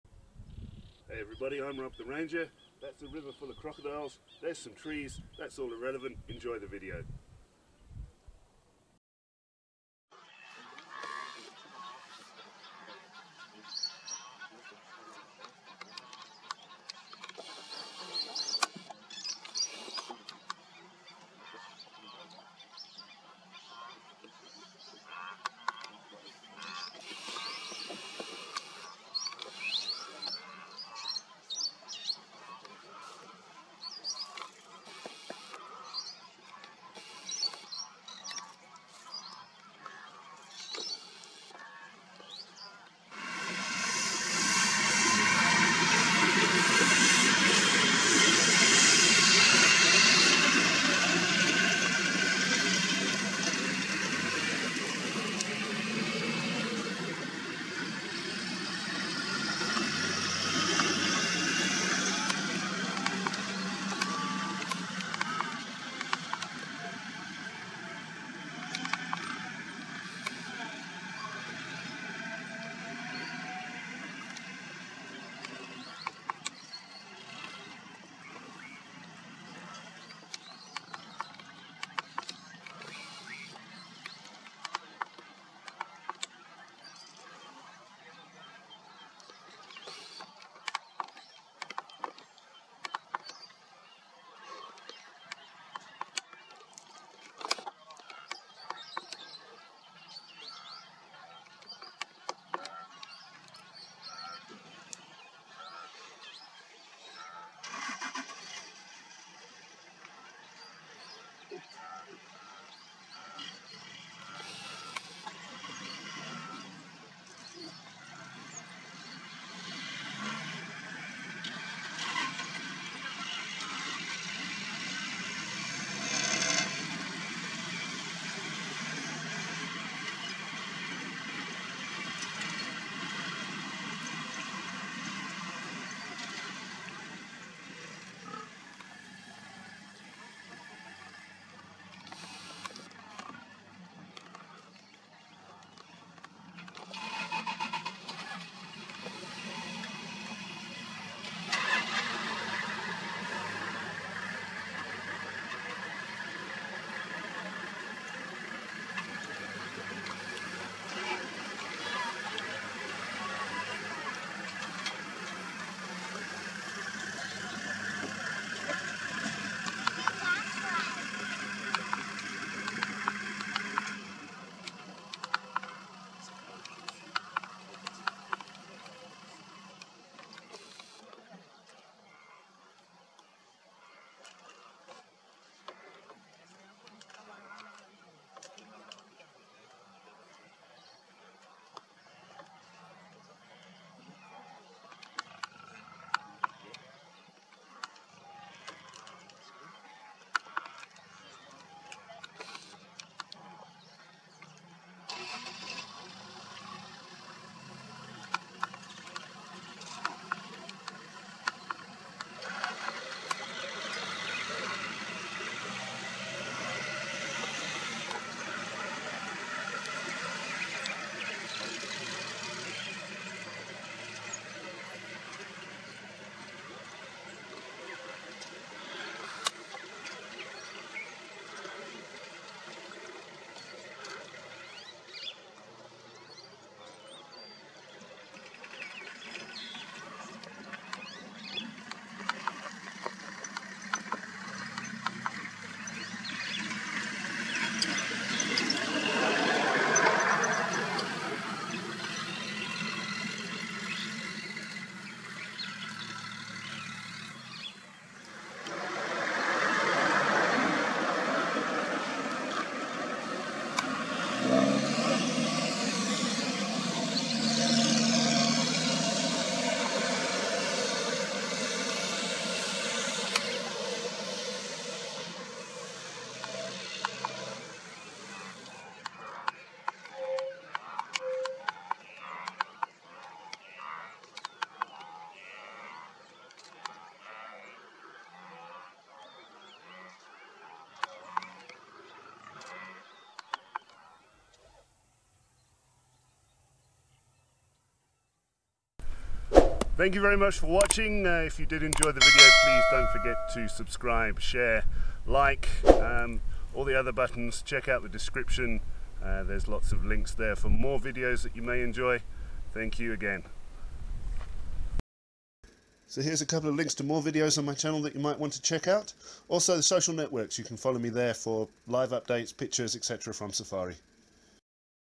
Lioness Is Not Ready To Mate! | Maasai Mara Safari | Zebra Plains
A Porini lioness is not quite ready to mate with the male lion called Olbarikoi, she would be ready after a few more days. The male knows this and was staying close by to ensure it would be him that got to mate first.